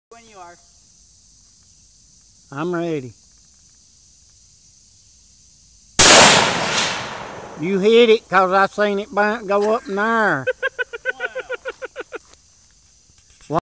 308 tracer round
308 tracer round hits a ½inch t1 steel plate